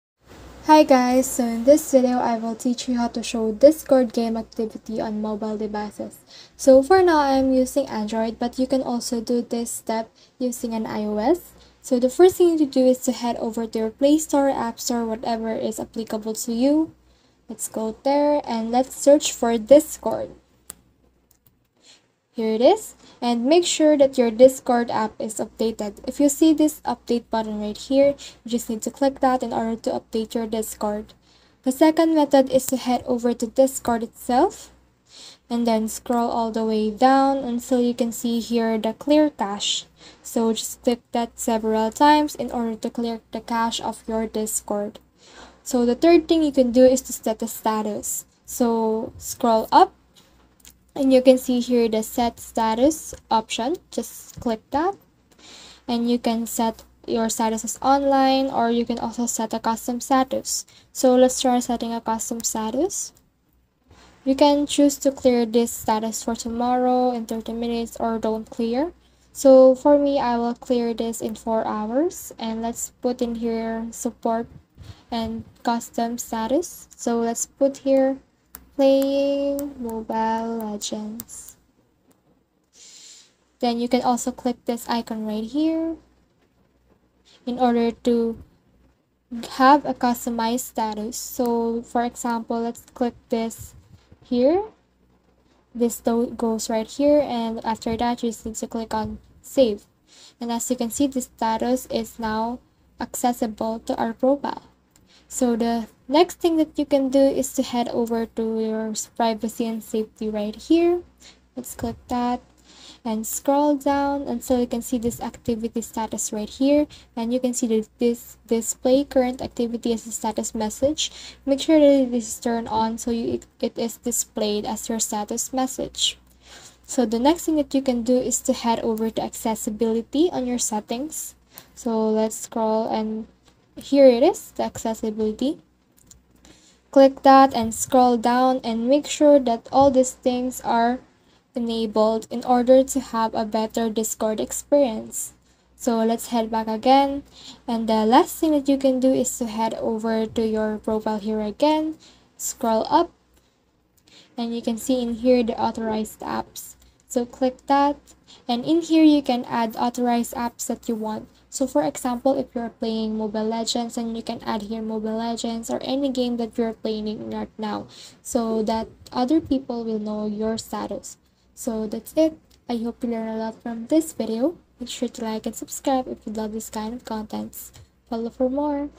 How To Show Discord Game Activity On Mobile Devices (2023)
In this video I'll show you How To Show Discord Game Activity On Mobile Devices. The method is very simple and clearly described in the video. Follow all of the steps & you will know how to show Discord Game activity on Mobile devices.